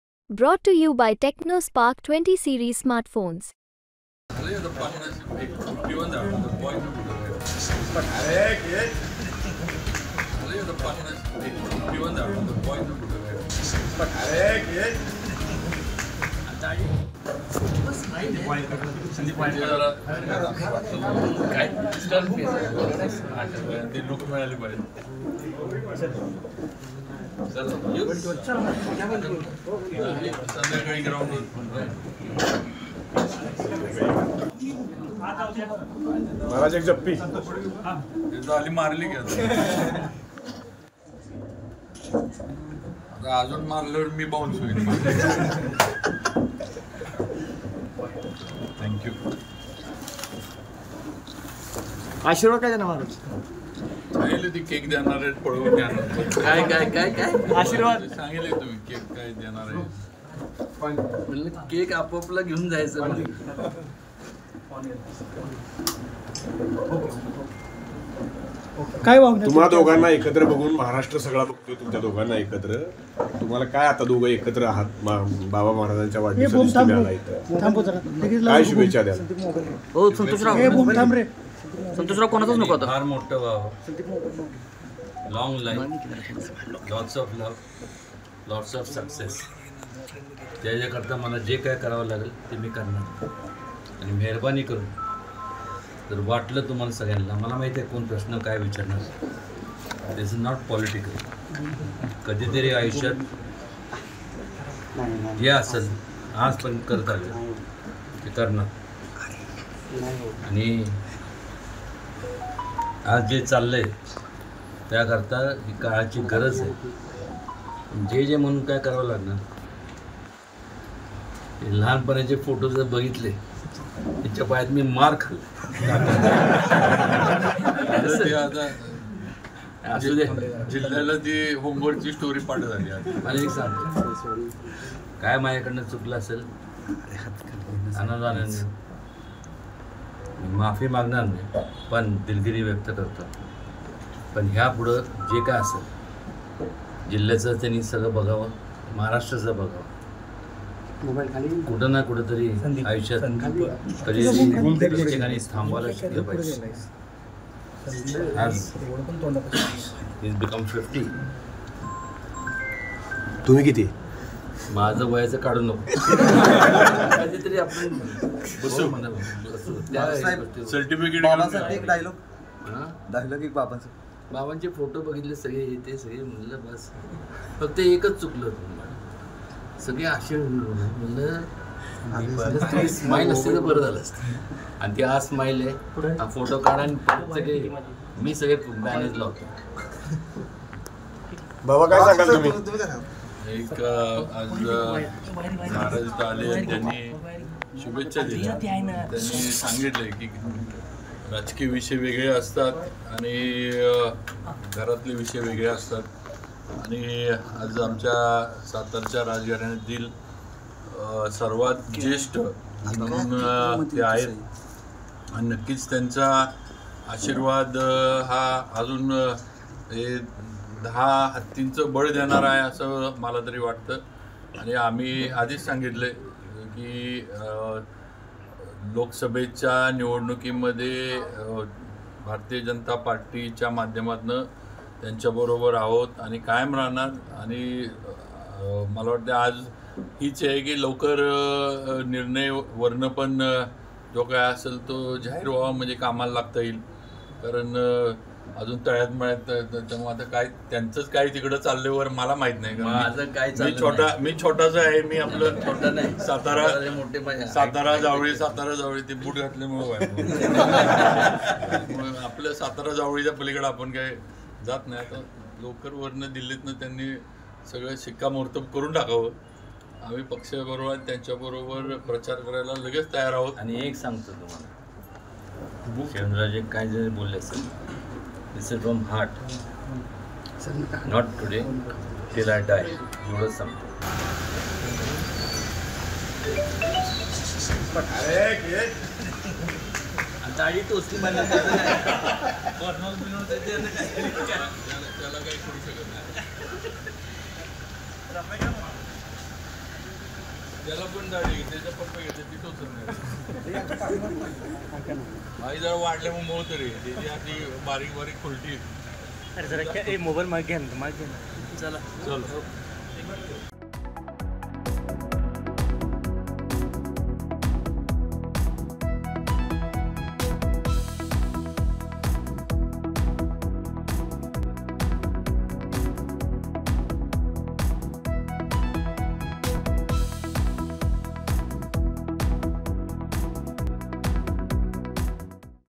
Udayanraje Bhosale यांनी शिवेंद्रराजे यांची भेट घेतली, मिठी मारली, दिलगिरीही व्यक्त, खुमासदार संवाद
खासदार उदयनराजे यांनी शिवेंद्रराजे यांची भेट घेतली, मिठी मारली, दिलगिरीही व्यक्त, खुमासदार संवाद.. वाढदिवसानिमित्त शिवेंद्रराजेंच्या भेटीचा हा प्रसंग खूपच चर्चेत आलाय.